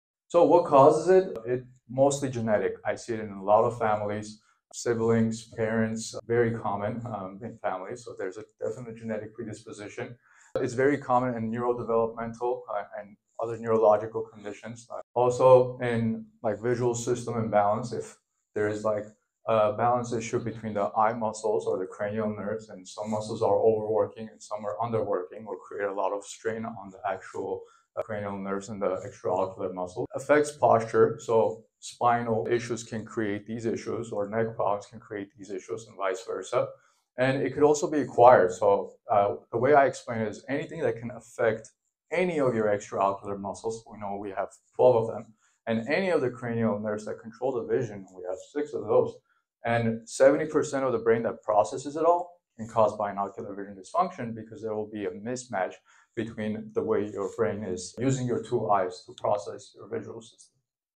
Part 3 of my lecture to our local optometric society—breaking down the root causes of Binocular
Part 3 of my lecture to our local optometric society—breaking down the root causes of Binocular Vision Dysfunction (BVD). It’s not just genetics or eye alignment. BVD can stem from head injuries, developmental delays, poor visual habits, prolonged screen time, or even postural imbalances. Many patients suffer for years without knowing their symptoms are vision-related. Vivid Visions Optometry, Inc. | Valencia, CA Virtual consultations & in-person exams available